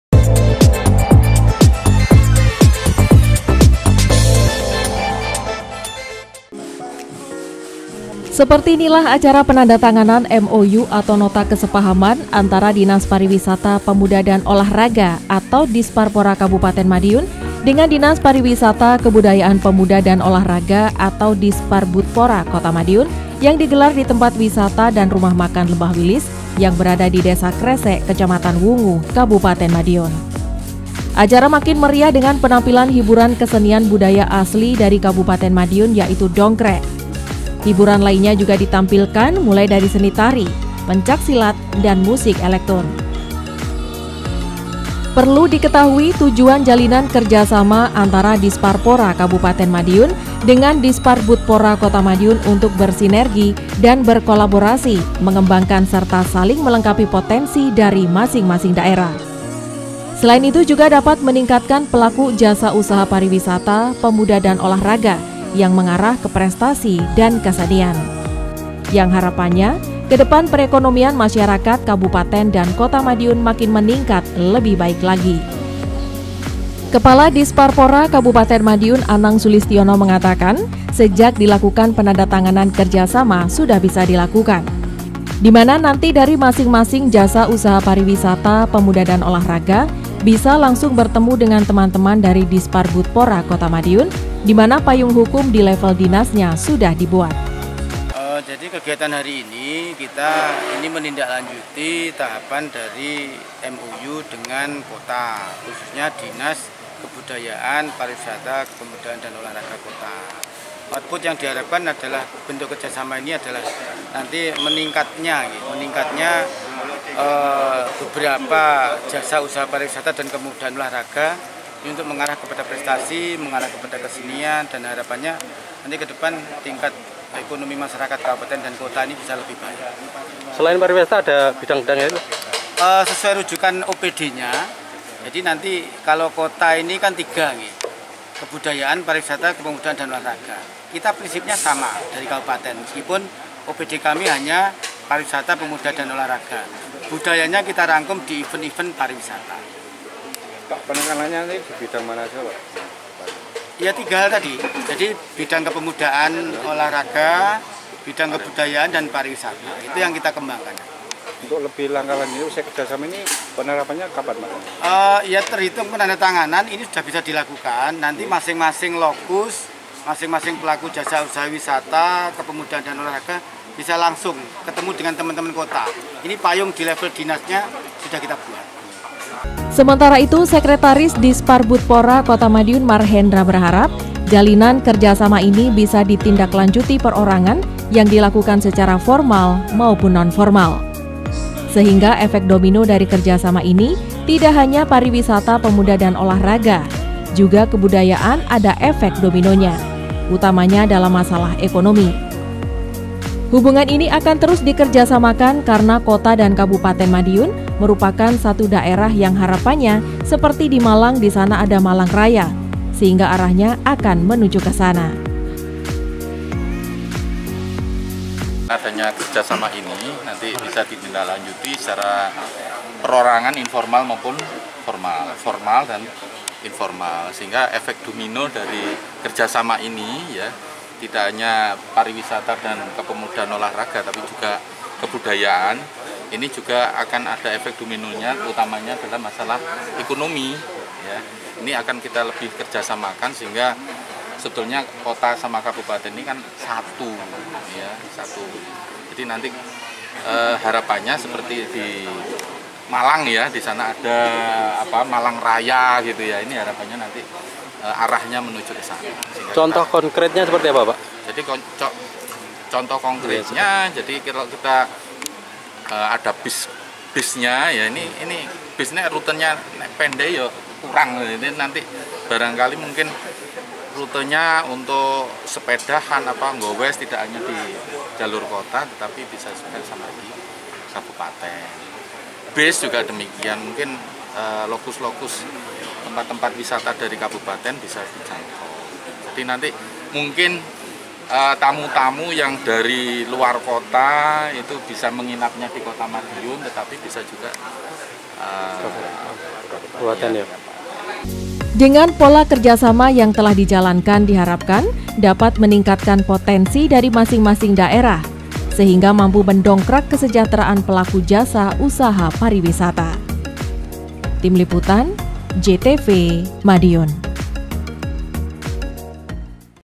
KABAR APIK - Kerjasama DISPARPORA KAB. Madiun & DISPARBUDPORA Kota Madiun
Seperti inilah acara penandatanganan mou atau nota kesepahaman antara dinas pariwisata pemuda dan olahraga atau disparpora kabupaten madiun dengan dinas pariwisata kebudayaan pemuda dan olahraga atau disparbudpora kota madiun yang digelar di tempat wisata dan rumah makan lembah wilis yang berada di desa kresek kecamatan wungu kabupaten madiun.